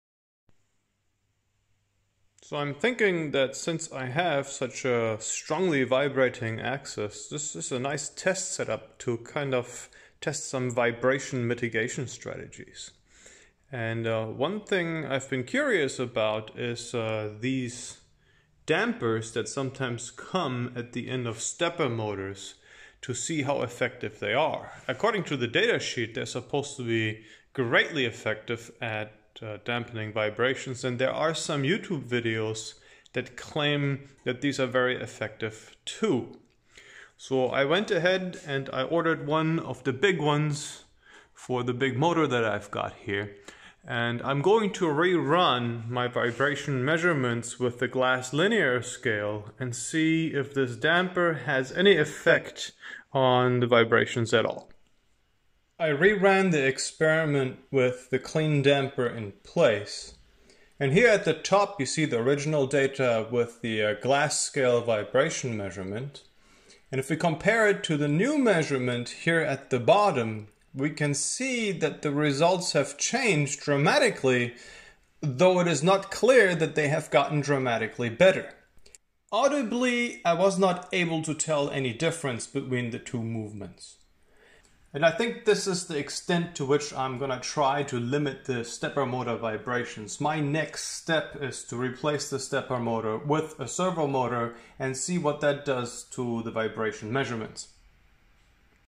Granite Surface Plate Based CNC Mill - CleanDamper Effect on Vibrations
I am testing the effect of adding a CleanDamper to the Y axis to see how it affects vibrations.